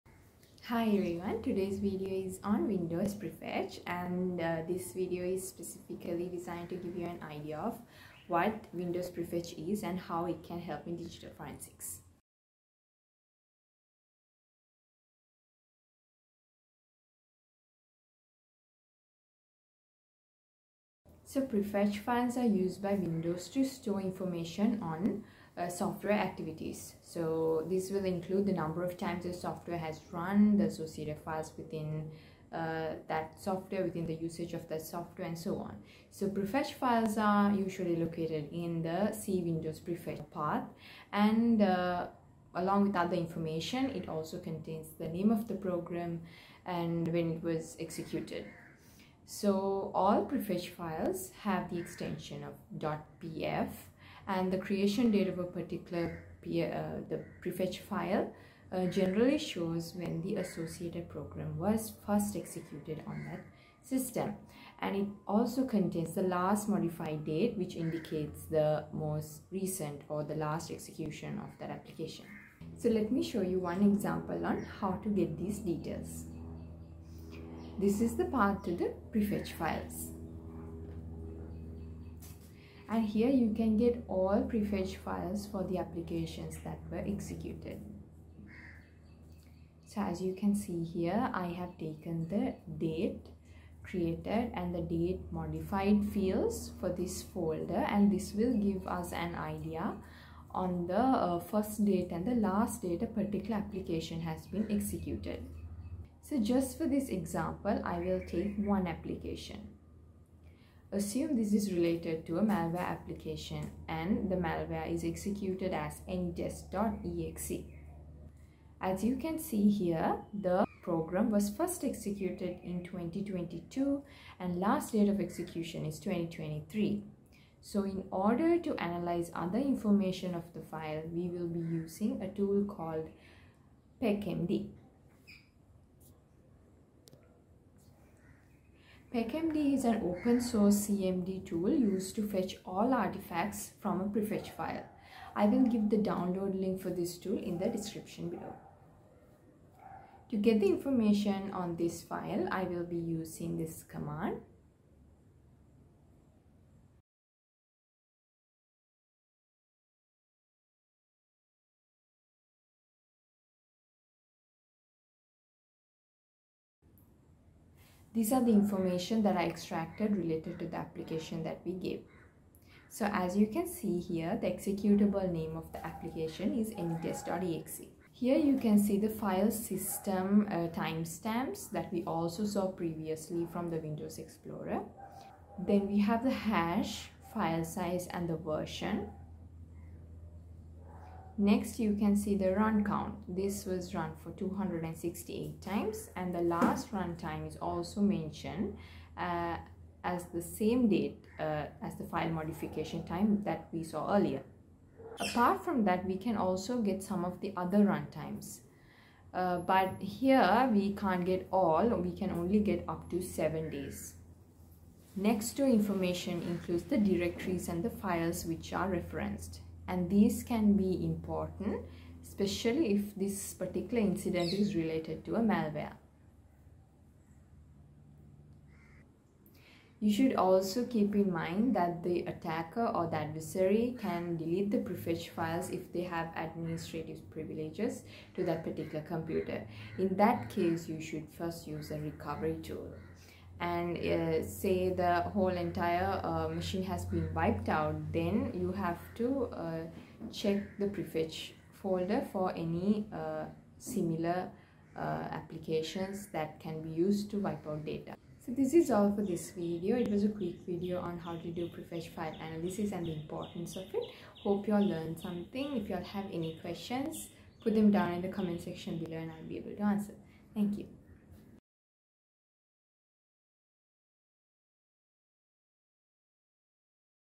How to use Windows Prefetch
Learn how to investigate digital activities using Windows Prefetch with this practical demo. See how it can reveal valuable clues about a computer's history and user actions.